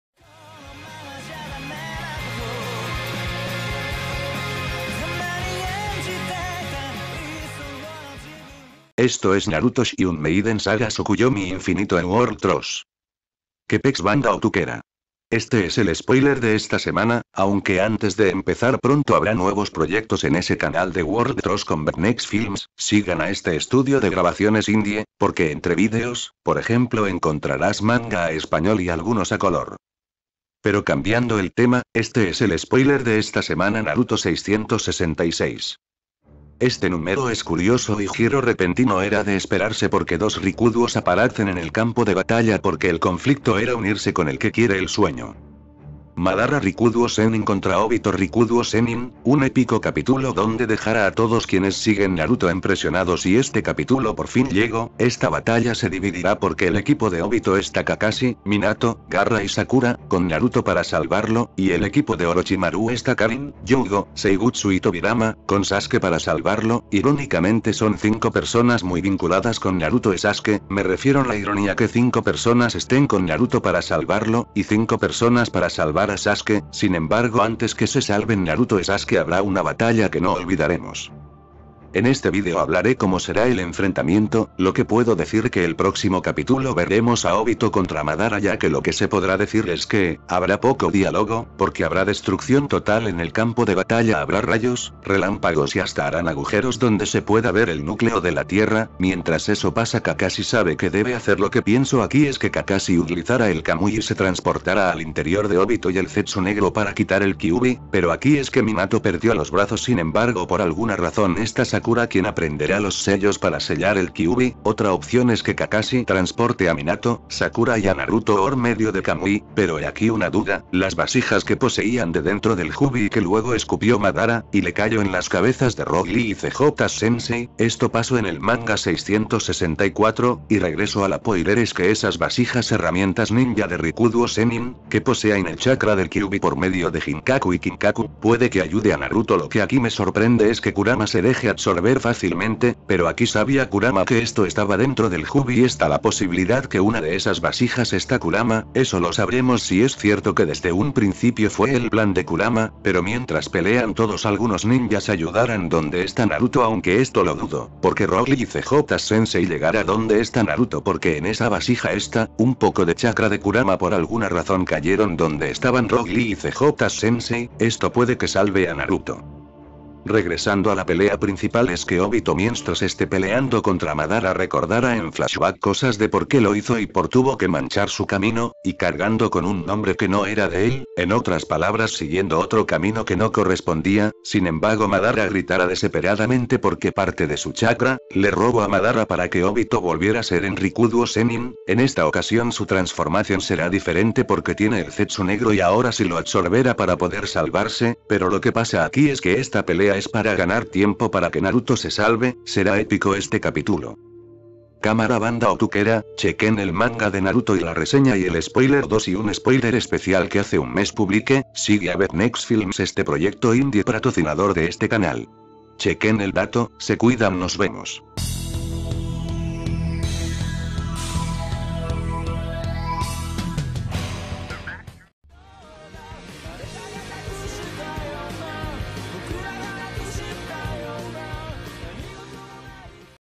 Naruto Manga 666 Spoiler 1 - Madara Rikuduo Vs Obito Rikuduo, Épico Batalla Infernal!!!
Obito Quiere Dejar Todo Claro, Porque Es Uchiha Obito y No Uchiha Madara,
Obito Obtendrá El Poder De Nuevo De Rikuduo Sennin, Mientras Esa Pelea,
Esta Destrtuyendo Todo, Kakashi Sabe Que Debe Utiliar El Kamui Para Sacar Al Kyubbi,y Minato Pronto Terminará Su Edo Tensei, Un Manga Épico Se Aproxima.

Mundo Del Ocio Anime, Manga, Games, Cosplay!!!